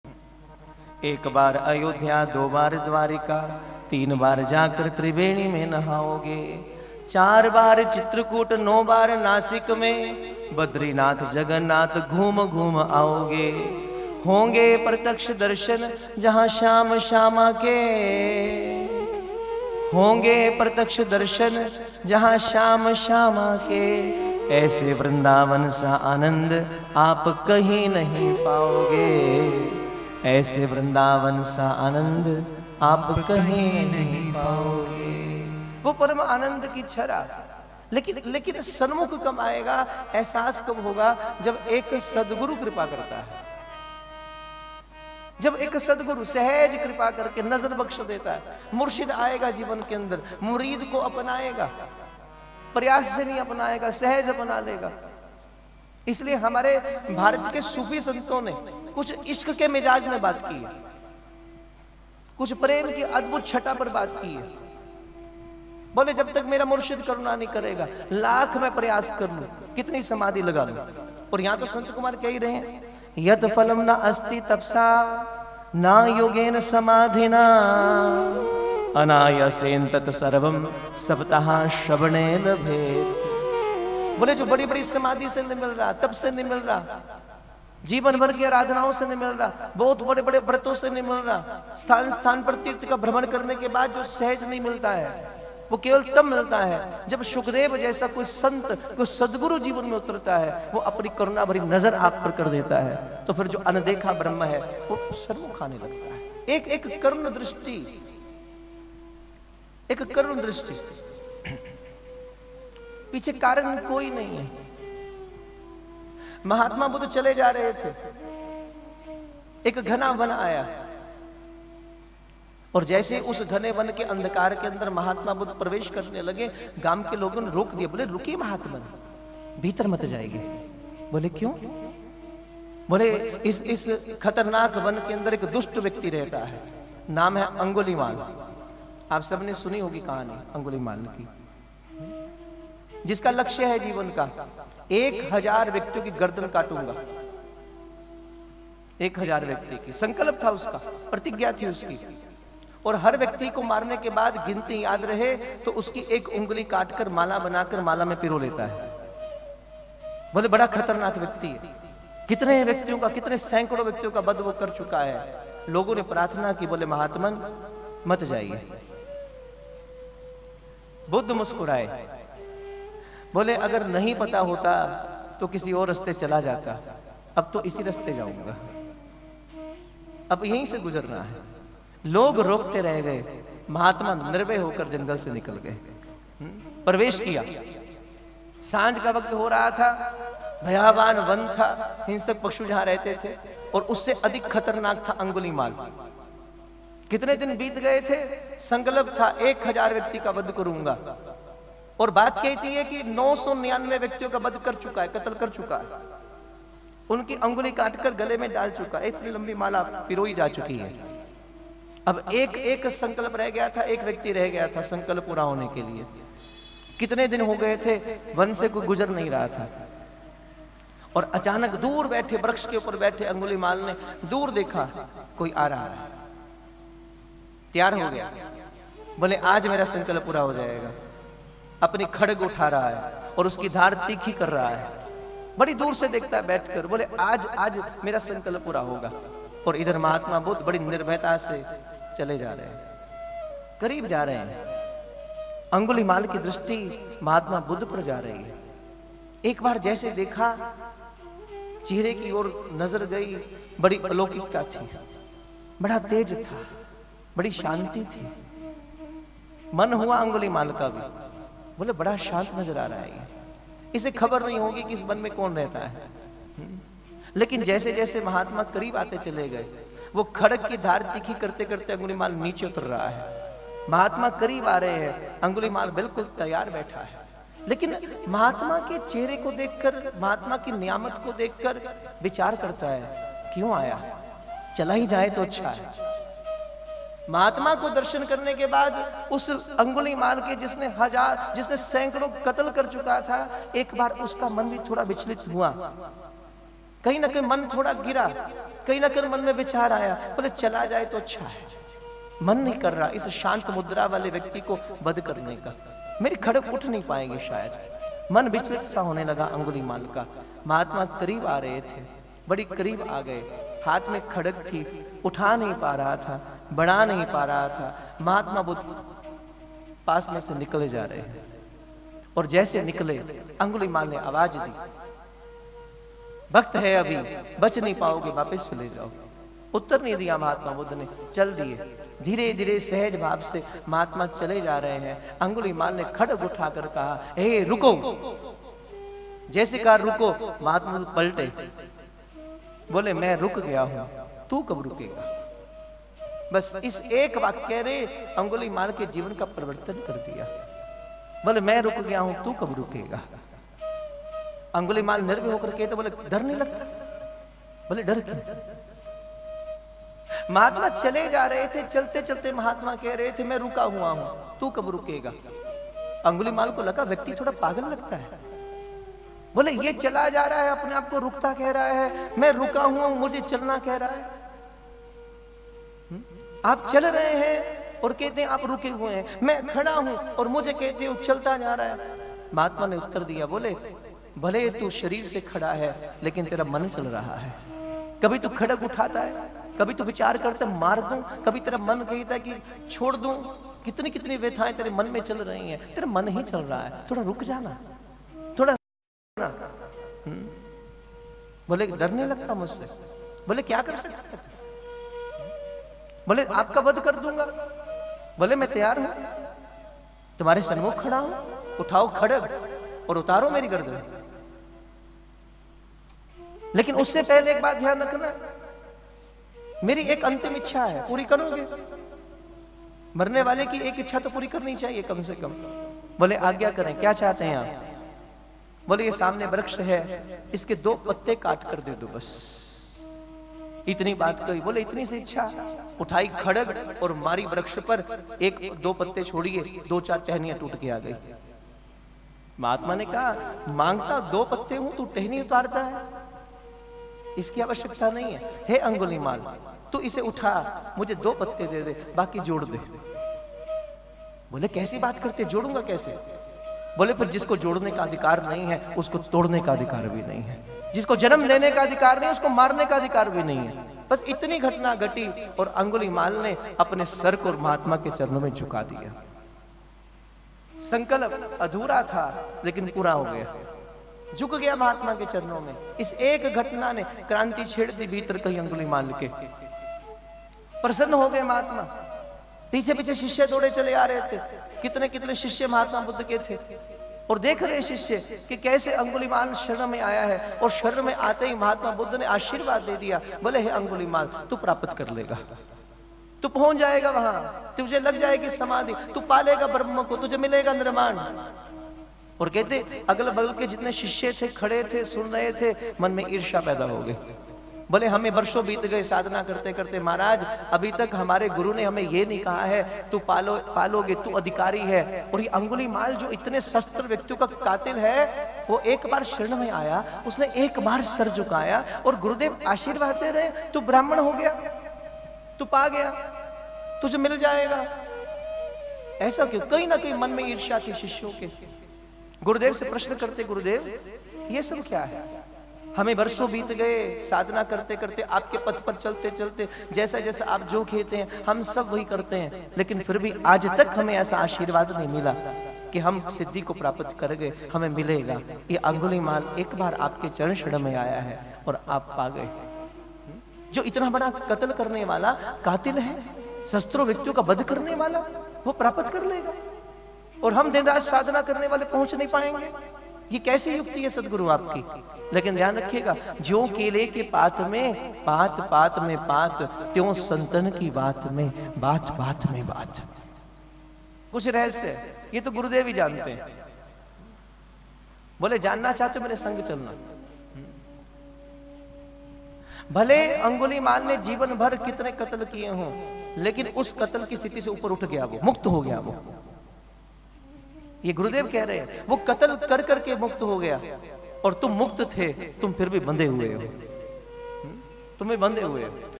डाकू अन्गुलिमाल के जीवन का बदलाव || Daku Angulimal Part 1 || सदगुरु की करुणा || Sri Sri Gurudev
डाकू अन्गुलिमाल के जीवन का बदलाव || सदगुरु की करुणा || Daku Angulimal Part 1
This is very inspirational video for all world at present time because today we are seeing many crimes at our surroundings and this video can change the mentality of any criminal or bad person.